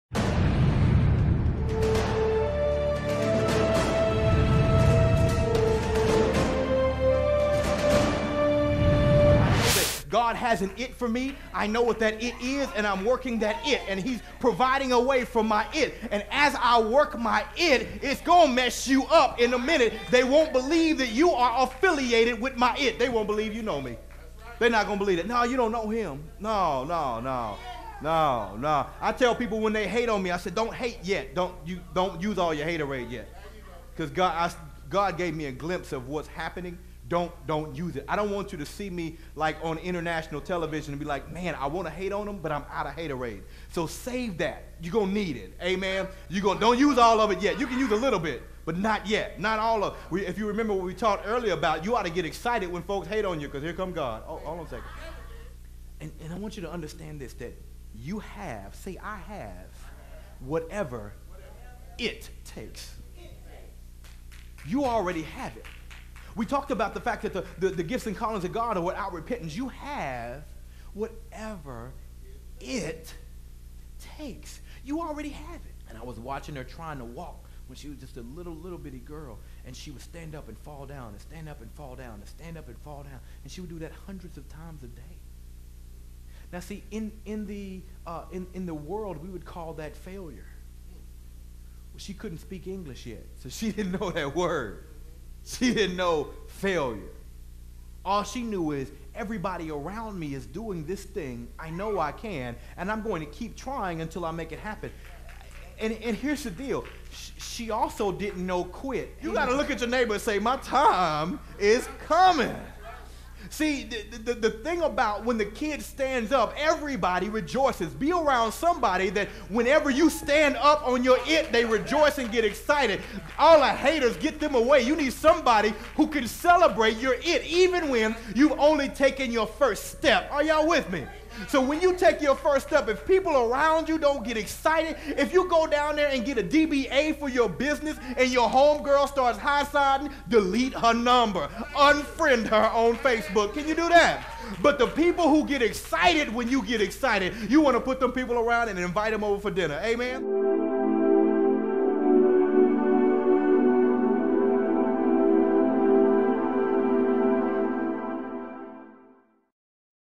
Lets Get it Started Highlight
Pastor Washington of the Love Church teaches on Let's get "IT" started. What is your "IT"? The Love Church is in Fort Worth TX,2622 N. Edgewood Terrace suite a fort worth TX. Services are every Sunday 9:30-10:29